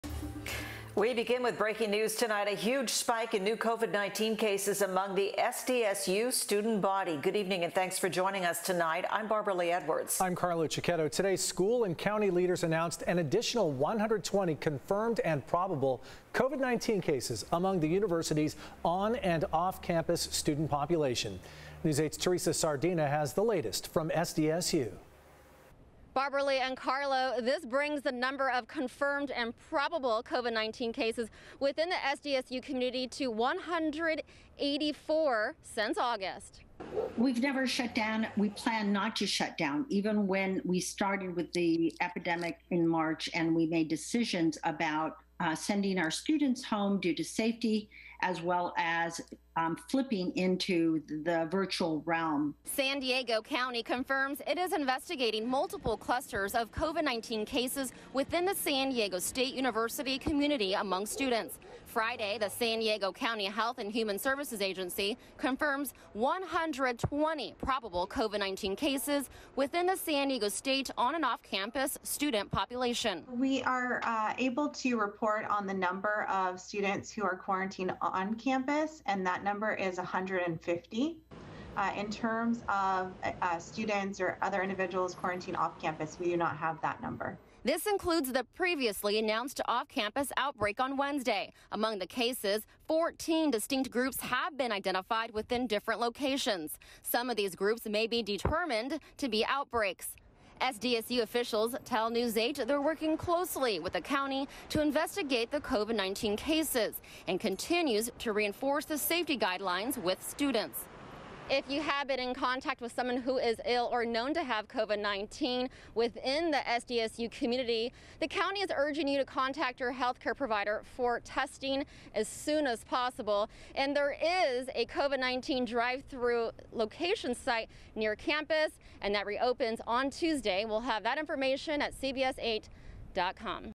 SDSU confirmed 120 cases Friday, 184 since August 24
In the last two weeks, 184 students tested positive for the virus. The County is investigating 14 clusters of the virus located around the College Area.